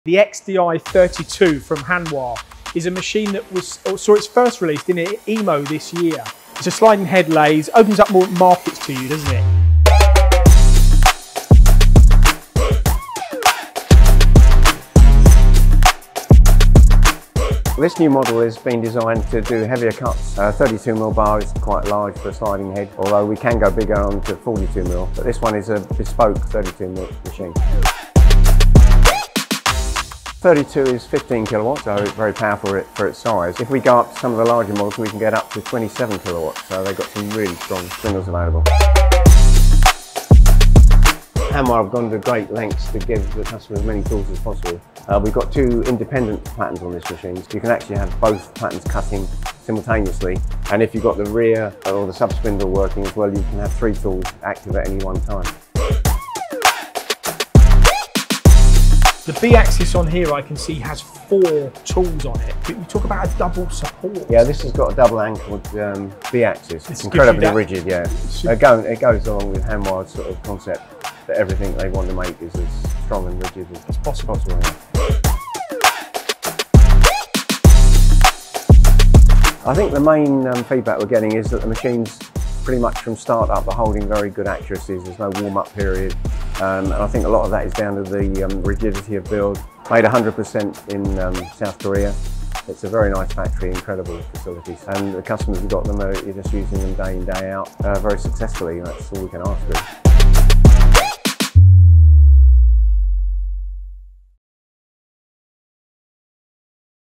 New 32mm sliding head from Hanwha
Introducing the cutting-edge Hanwha XDI 32 machine – a powerhouse of precision with robust machining performance and unparalleled productivity. Equipped with advanced features including Fanuc/Siemens control, a sub-spindle, 6,500rpm capability, and a maximum turning diameter of 32mm, this machine stands at the forefront of innovation. With up to 26 tools and multiple axes, including a double supported B axis, it offers versatility for intricate machining tasks. Paul, journeyed to Hove to engage with Eric Dugard and delve deeper into the intricacies of this new model.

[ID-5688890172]